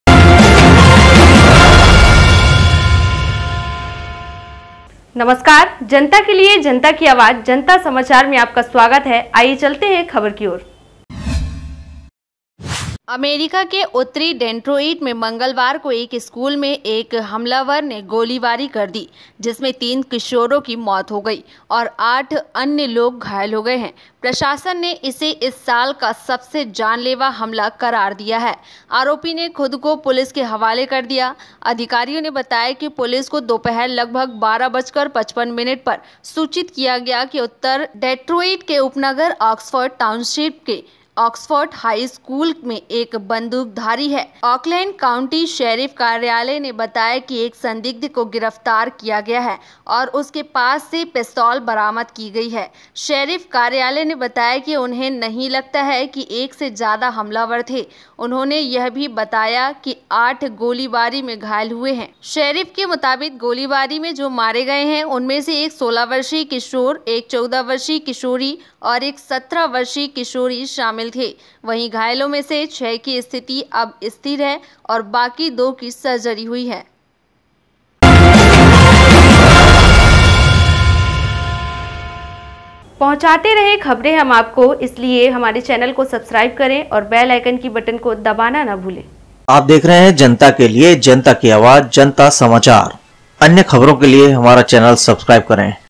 स्कूल में हमलावर ने की गोलीबारी America, 1/12/2021 JantaSamachar.News#HindiNewsChannel
NewsInHindiSamachar,
Janta samachar,
The attacker opened fire in the school,
स्कूल में हमलावर ने की गोलीबारी,

अमेरिका के उत्तरी डेट्रोइट में मंगलवार को एक स्कूल में एक हमलावर ने गोलीबारी कर दी जिसमें 3 किशोरों की मौत हो गई और आठ अन्य लोग घायल हो गए हैं। प्रशासन ने इसे इस साल का सबसे जानलेवा हमला करार दिया है। आरोपी ने खुद को पुलिस के हवाले कर दिय है।

अधिकारियों ने बताया कि पुलिस को दोपहर लगभग 12 बजकर 55 मिनट पर सूचित किया गया कि उत्तर डेट्रोइट के उपनगर ऑक्सफॉर्ड टाउनशिप के ऑक्सफॉर्ड हाई स्कूल में एक बंदूकधारी है। ऑकलैंड काउंटी शेरिफ कार्यालय ने बताया कि एक संदिग्ध को गिरफ्तार किया गया है और उसके पास से पिस्तौल बरामद की गई है। शेरिफ कार्यालय ने बताया कि उन्हें नहीं लगता है कि एक से ज्यादा हमलावर थे। उन्होंने यह भी बताया कि आठ गोलीबारी में घायल हुए हैं।

शेरिफ के मुताबिक, गोलीबारी में जो मारे गए उनमें से एक 16 वर्षीय किशोर, एक 14 वर्षीय किशोरी और एक 17 वर्षीय किशोरी शामिल थे। वहीं, घायलों में से छह की स्थिति अब स्थिर है और बाकी दो की सर्जरी हुई है।

An attacker opened fire on a school in northern Detroit on Tuesday, killing three teenagers and injuring eight others. The administration has termed it as the deadliest attack of this year. The accused has handed himself over to the police.

Officials said police were notified at about 12:55 p.m. that a gunman was at Oxford High School in Oxford Township, a suburb of north Detroit. The Auckland County Sheriff's Office said a suspect had been arrested and a pistol was recovered from him. The sheriff's office said they did not believe there was more than one attacker. He also said that eight were injured in the firing.

According to the sheriff, a 16-year-old teenager, a 14-year-old and a 17-year-old were among those killed in the shootout. At the same time, the condition of six of the injured is now stable and the remaining two have undergone surgery.